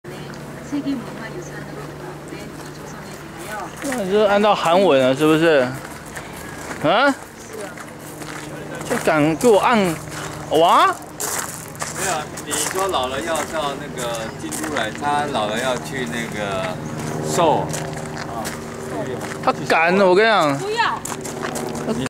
2013-06-23 京都二条城內
用小石子鋪路的原因是容易排水及有人走過時會發出聲響,防止忍者或刺客